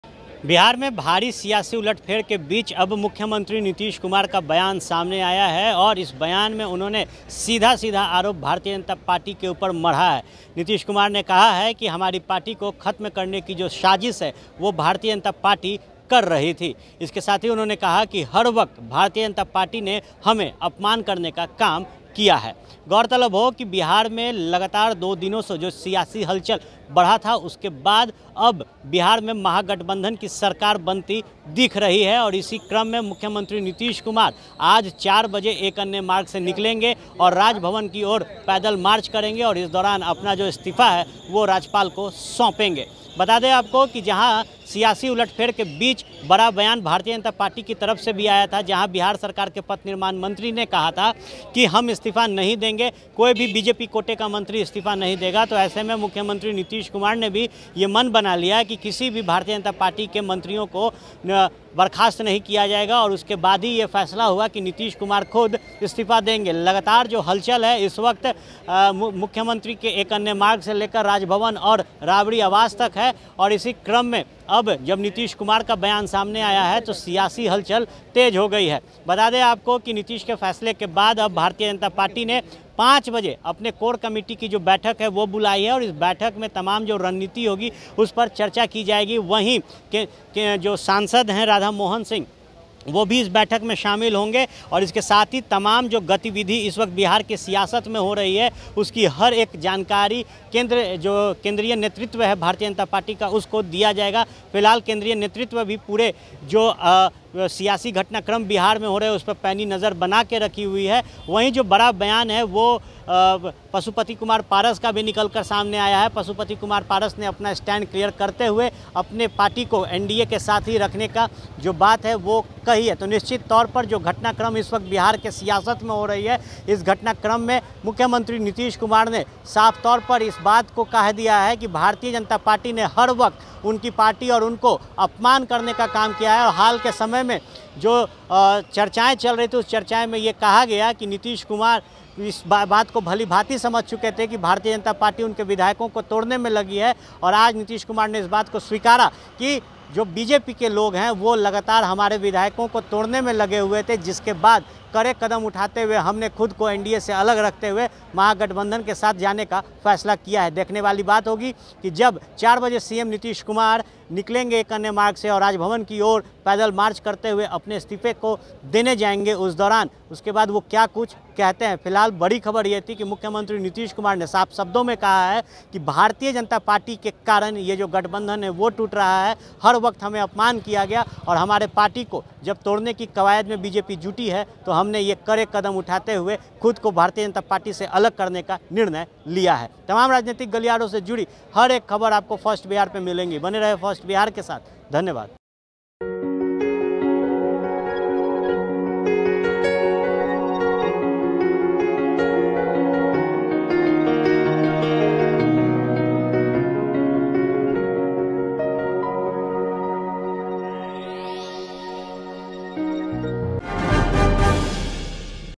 JDU बैठक के बाद Nitish का बड़ा बयान, पार्टी को बर्बाद करने की थी तैयारी
3.JDU को डूबता नाव बनाने के प्रयास में थे RCP Singh अब जदयू तेज रफ्तार से जहाज बनेगा